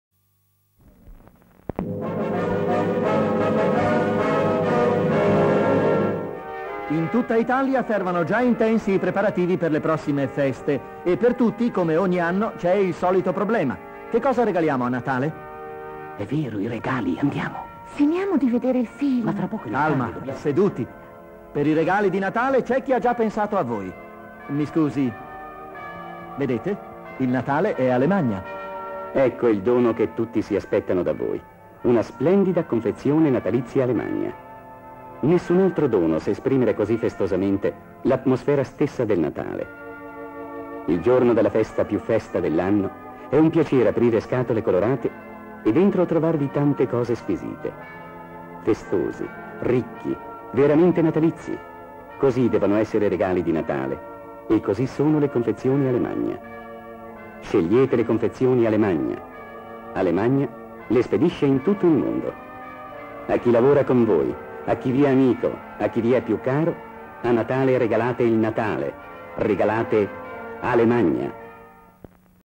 Confezioni natalizie Alemagna Mondo attualità
Spot Confezioni natalizie Alemagna Mondo attualità, Anno prima messa in onda 1968.